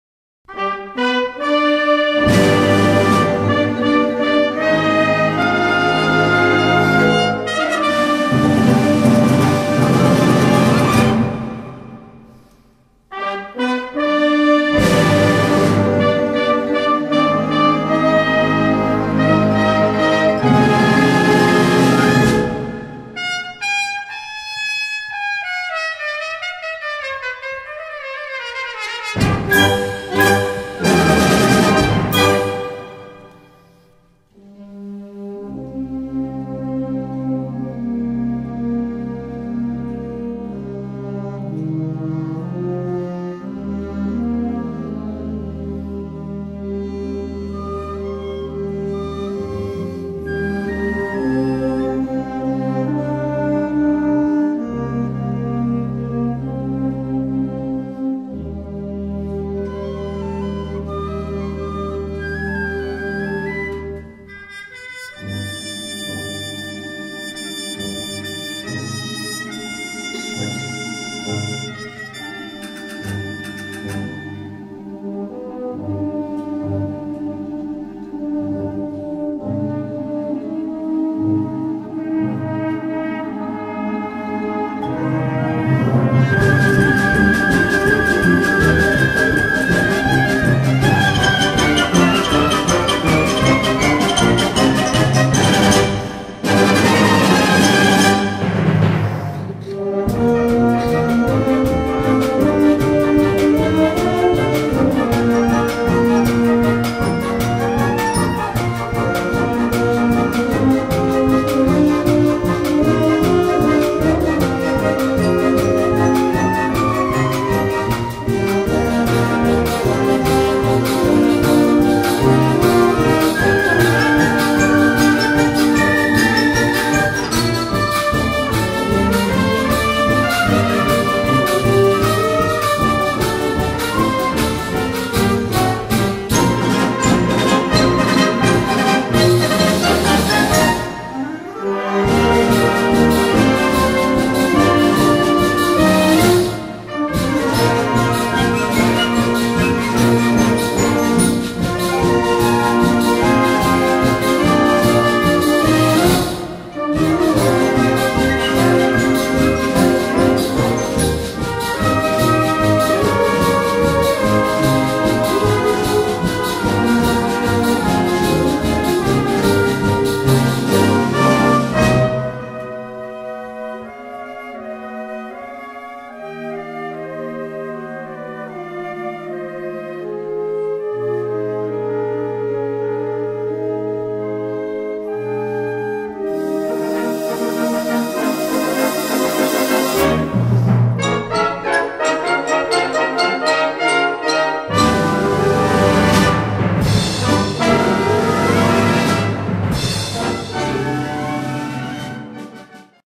5 Dezember
Der musikalische Adventkalender der TK Kirchheim i.I gestattet den Einblick in einen kleinen Teil der vereinsinternen Bildarchive sowie die Veröffentlichung einiger Aufnahmen von einigen Musikstücken, die in den letzten 30 Jahren im alljährlichen Frühjahrskonzert dargeboten wurden.